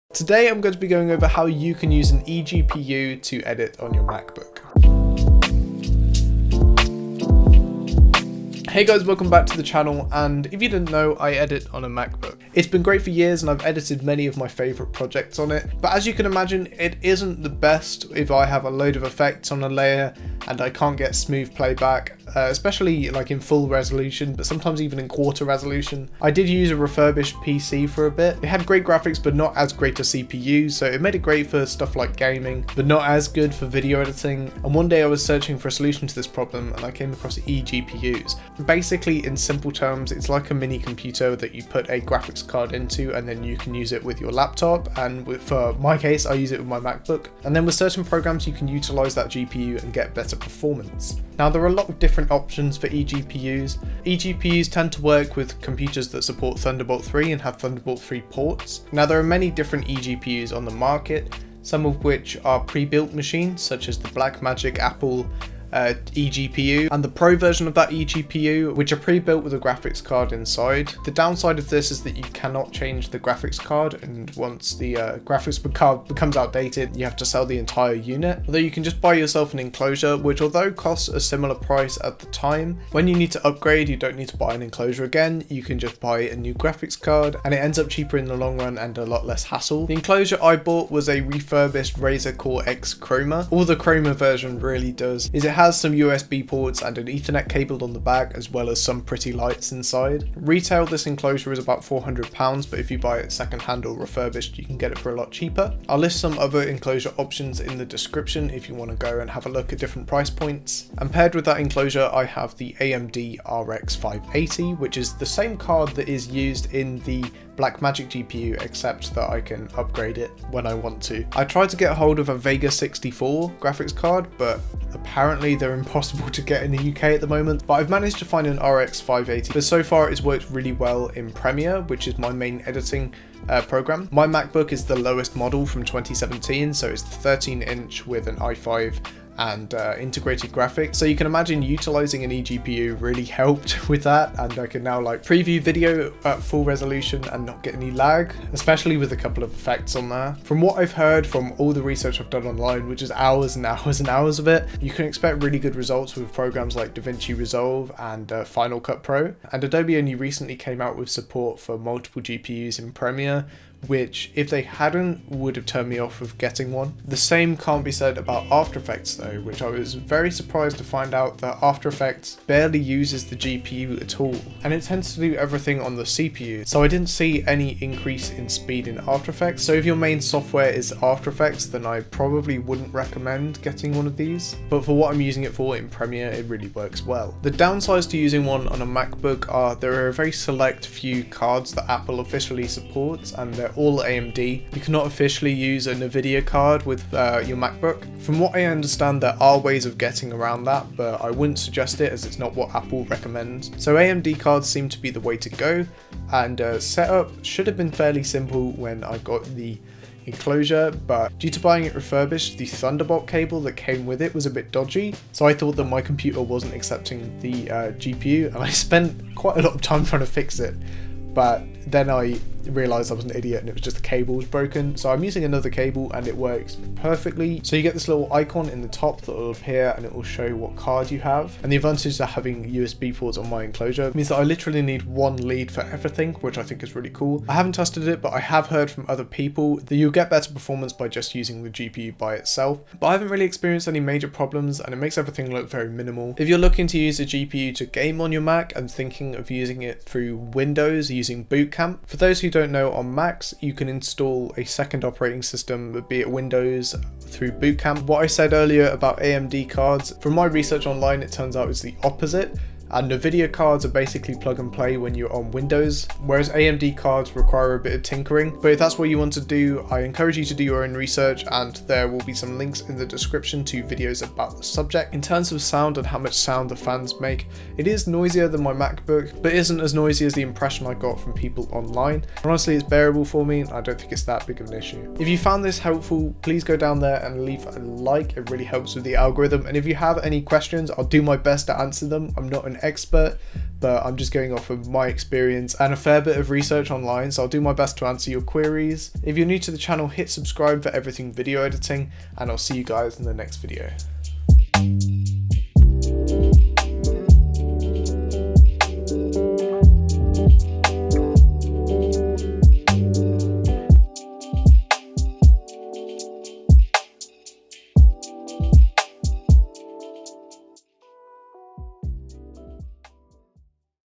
Edit Video Fast on a Macbook (eGPU for editing in 2021?)
Today I go over my switch to an eGPU, how it has sped up my editing process, the pros and cons of different setup and an overview of the subject.

My Enclosure:
Razer Core X Chroma

OTHER OPTIONS:
Apple Blackmagic eGPU: